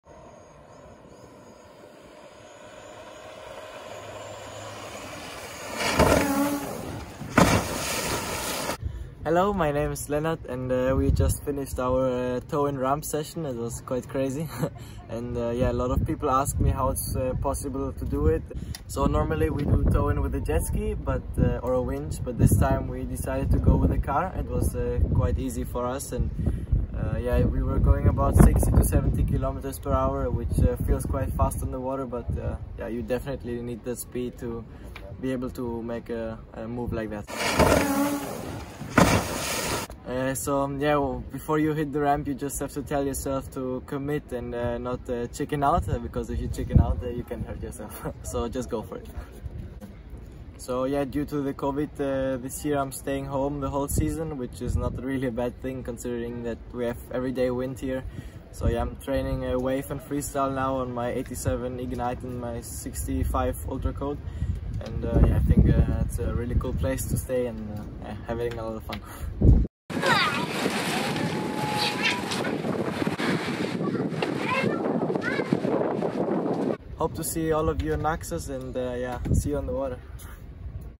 Starboard's Dream Team: Lennart Gets Pulled By A Car
Would you try being towed behind a car on your windsurf gear in 70 km/h and go full speed off a wooden ramp in 30 cm deep water? At the same time do a double rotation?

Greek/German young talent and Starboard Dream team rider Lennart Neubauer did it! He tells us what you need to really go for it and be committed. If not you will hurt yourself! Watch the clip and find out what went through his mind right before hitting the ramp.